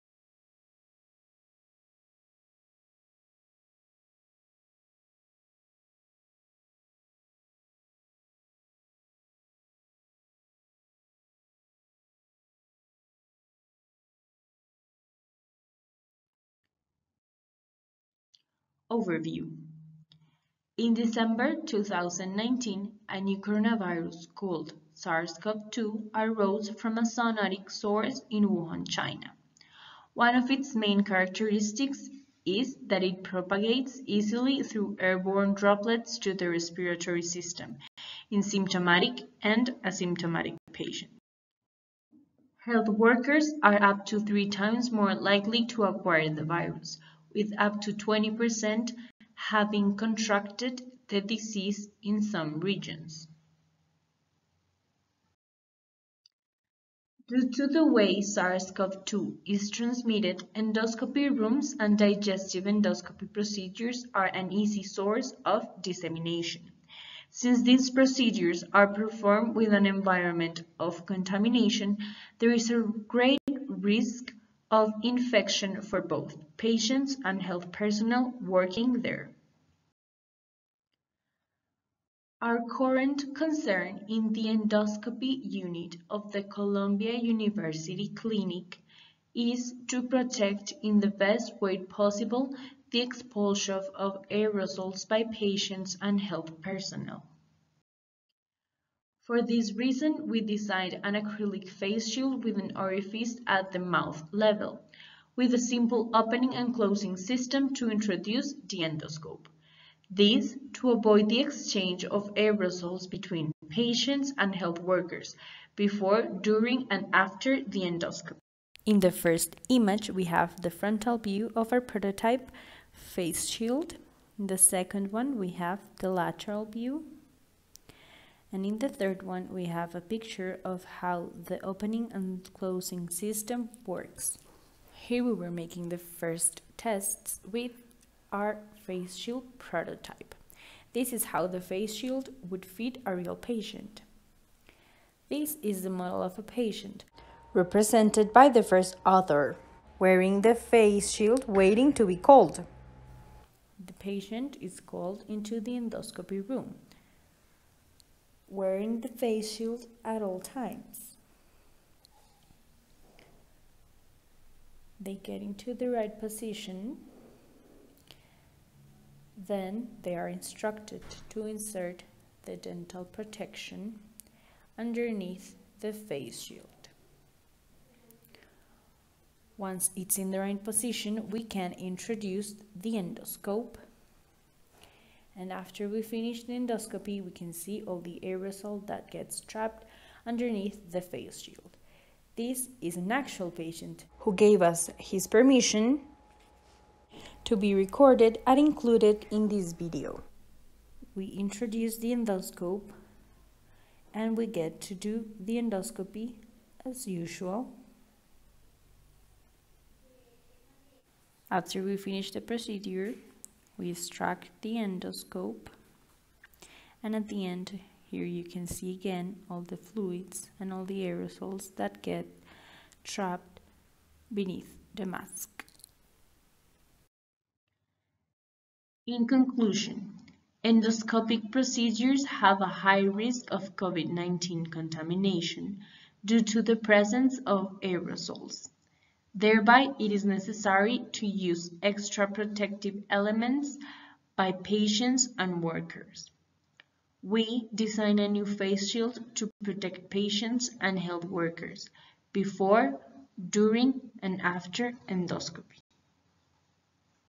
Use of a new face shield for patients of the endoscopy unit to avoid aerosol exchange
Diego et al demonstrate a new face shield for use in the COVID-19 era.